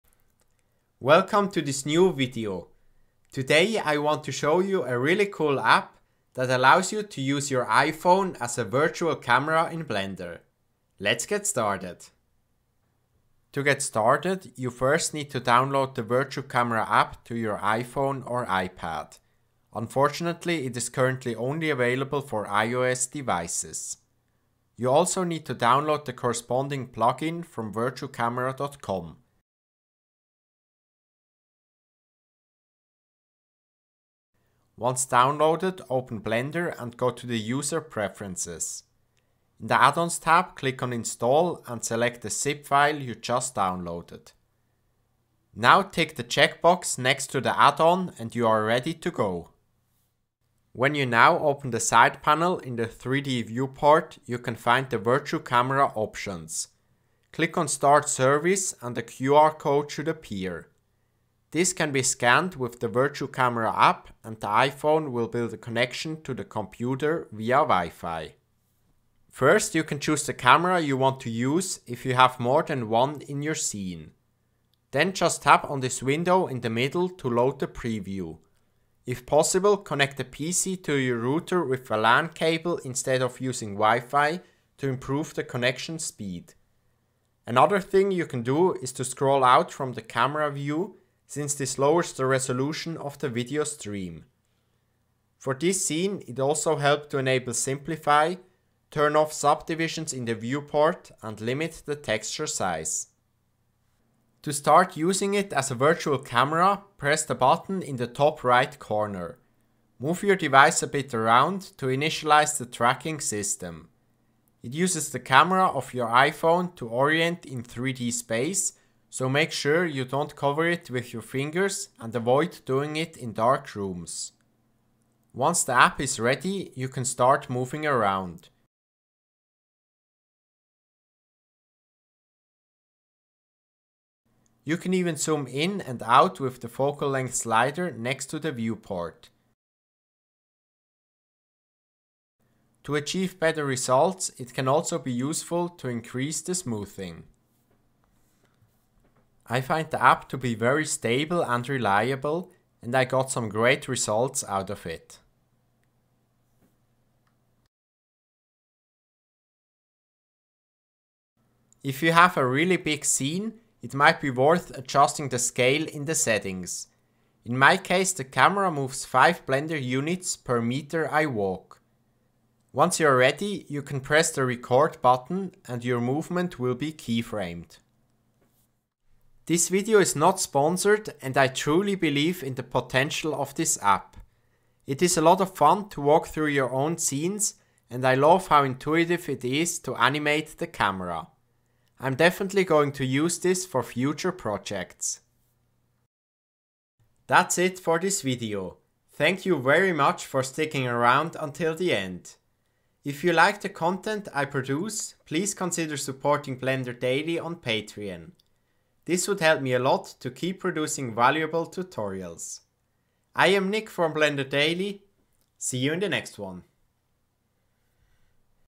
Virtual Camera in Blender with your Smartphone!
In this video I want to demonstrate VirtuCamera, an app, that allows you to control the Blender camera in real time with an iPhone or iPad. This is extremely useful to create realistic camera animations in a very intuitive way!